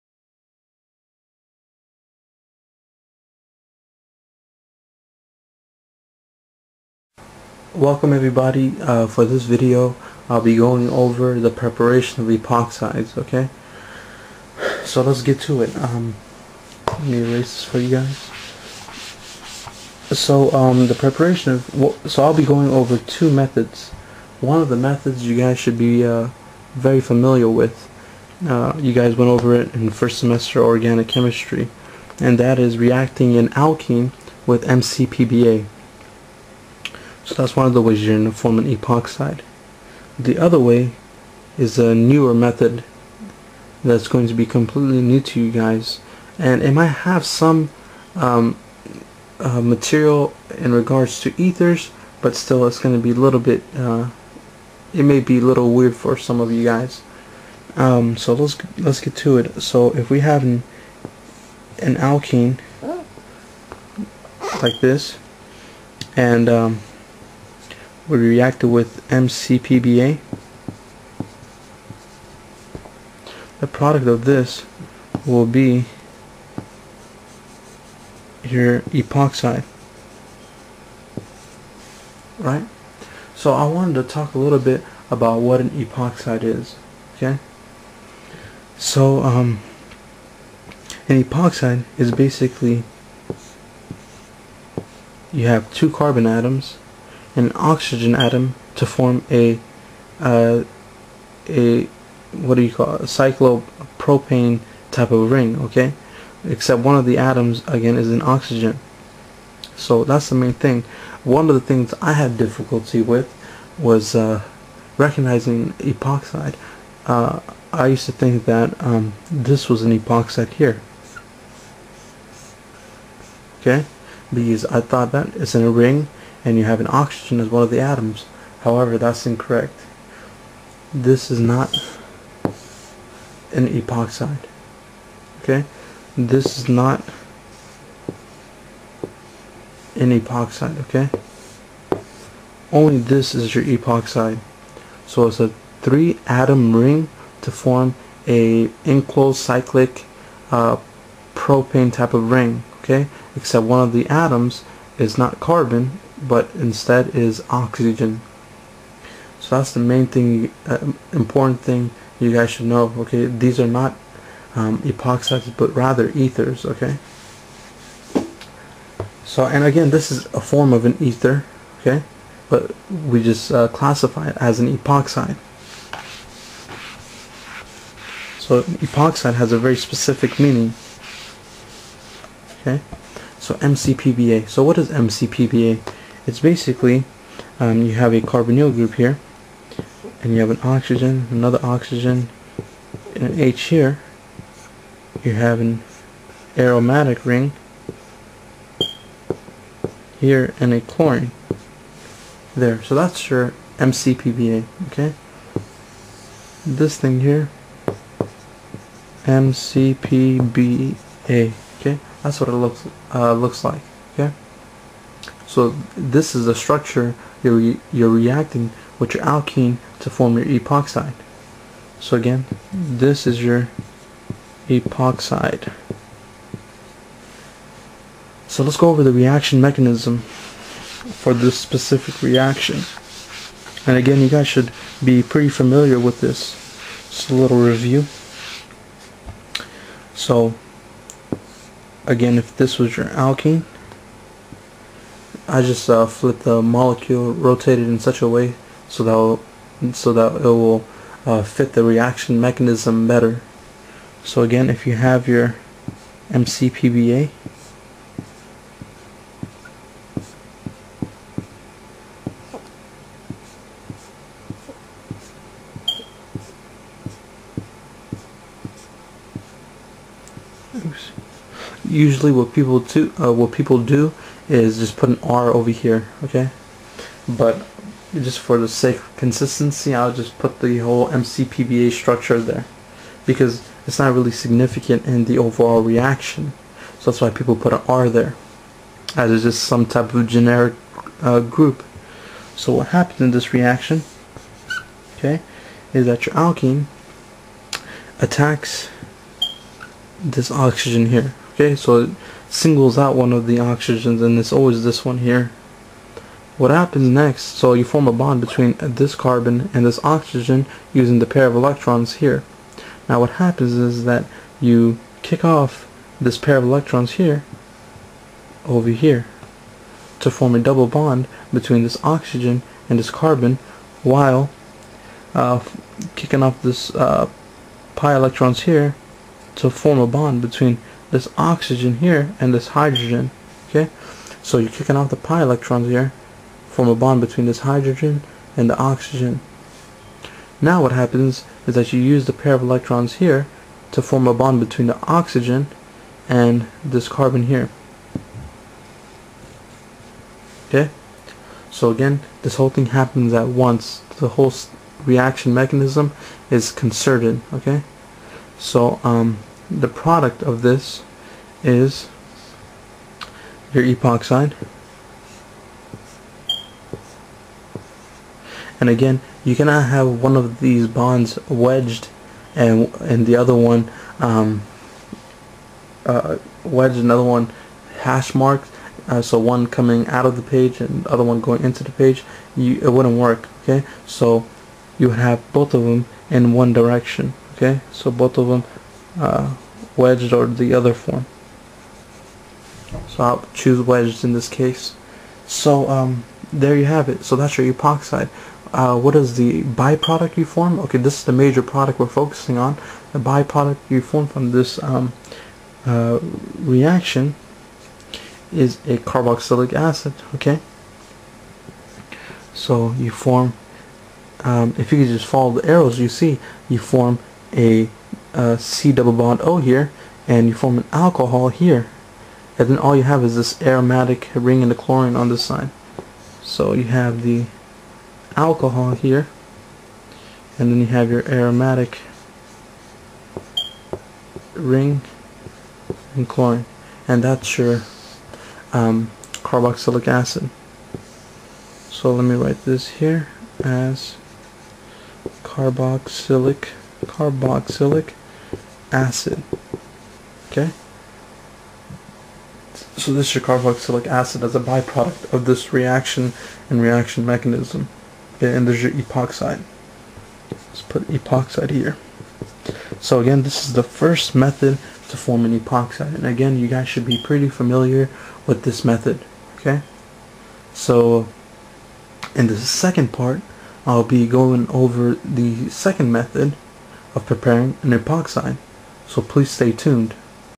Preparation of Epoxides part 1/2
Brief discussion on the preparation of epoxides

part 1/2